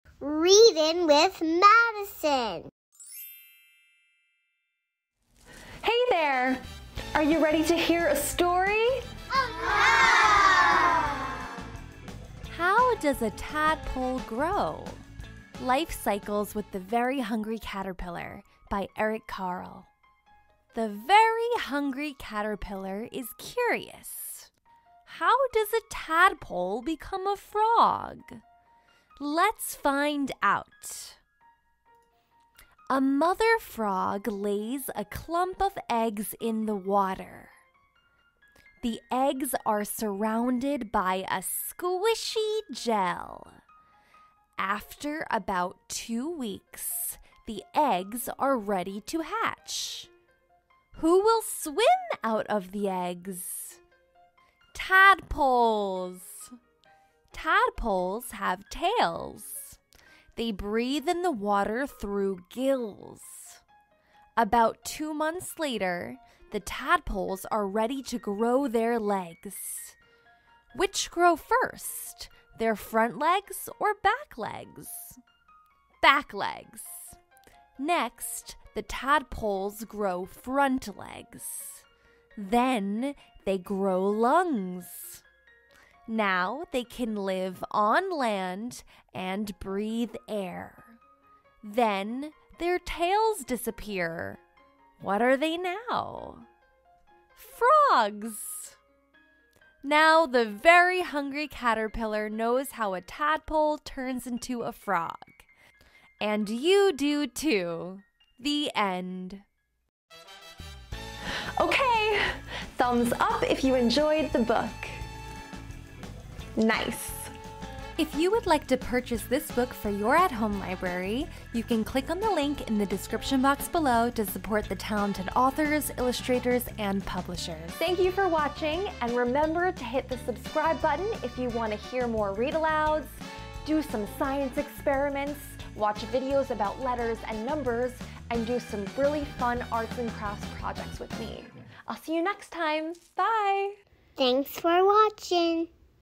How Does a Tadpole Grow? by Eric Carle || Lifestyles with the Very Hungry Caterpillar
Kids Book Read Aloud: How Does a Tadpole Grow? by Eric Carle || Lifestyles with the Very Hungry Caterpillar

You can purchase the book by clicking on this

Madison

** "Reading with Madison" VO by Benni Zoe Stewart **